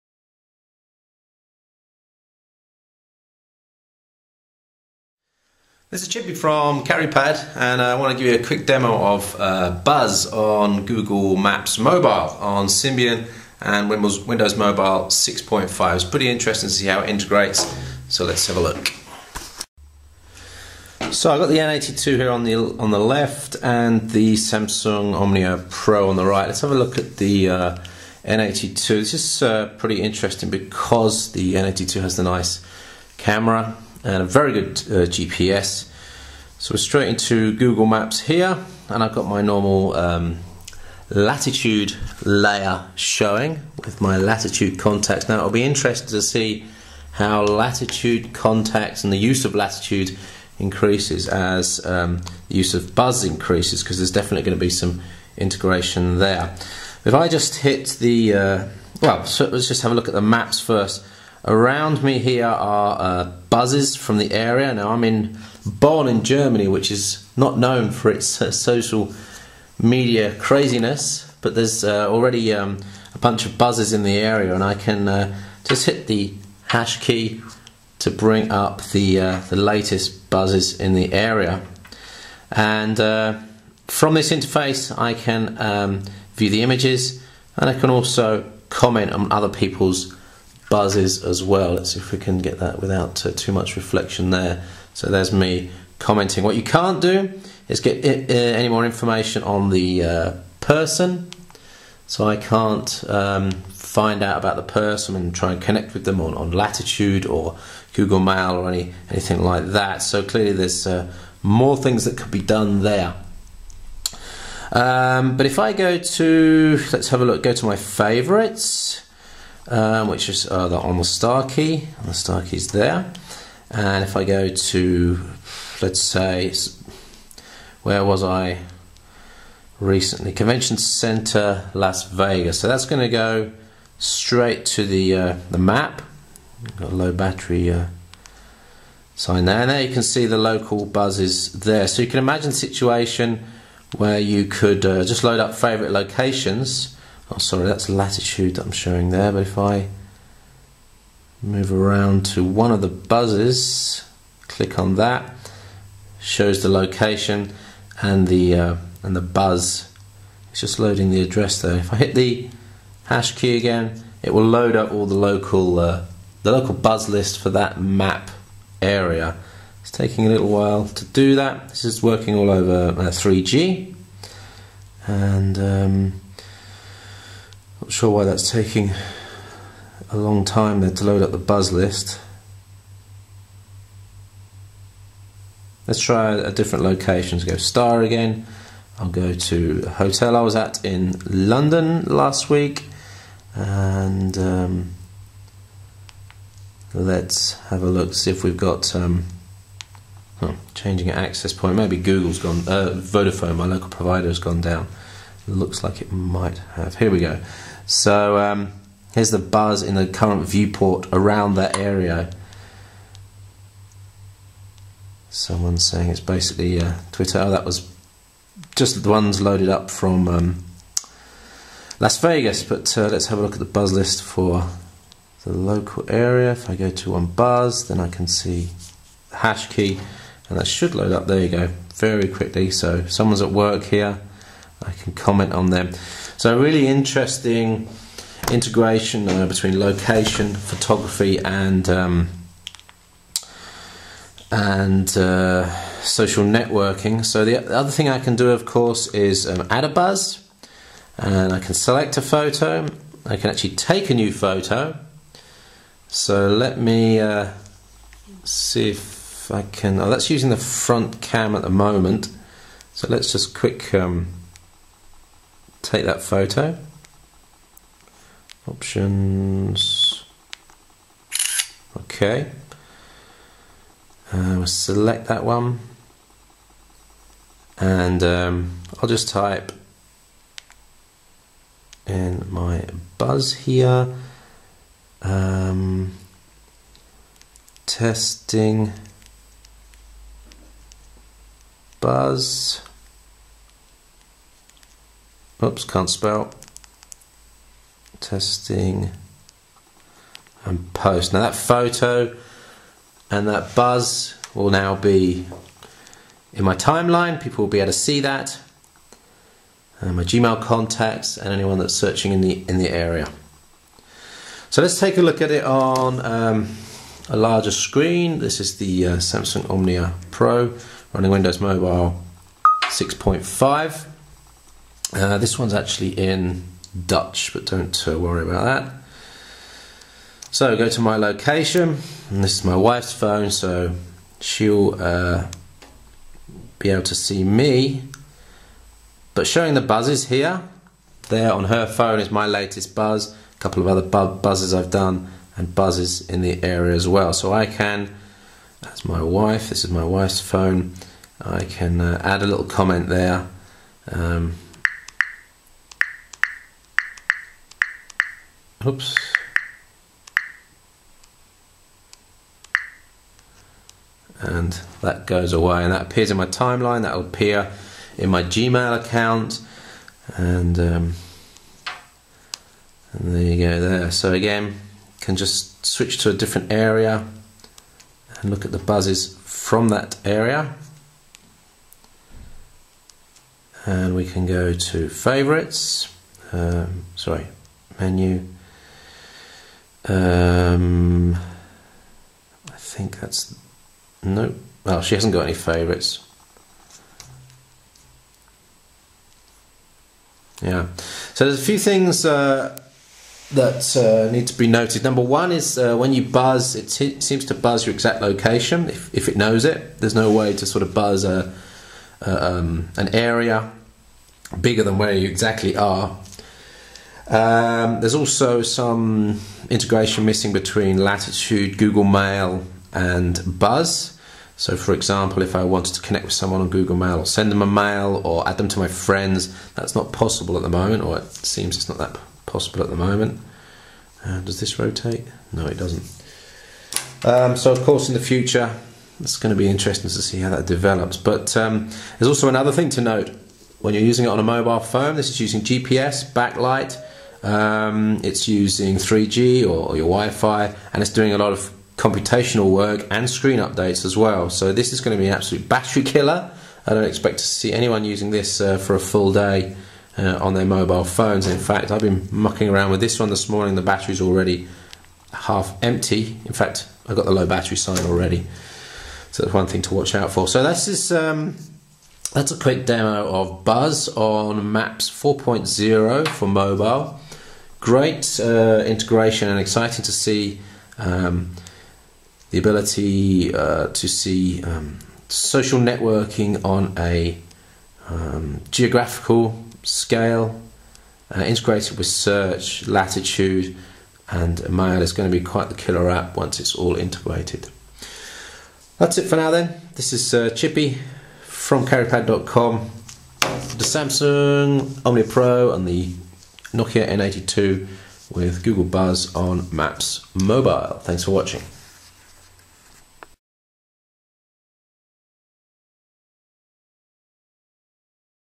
Google Buzz on Mobile Maps (Symbian and WM6.5)
testing Google Buzz integration on Google Maps for Mobile.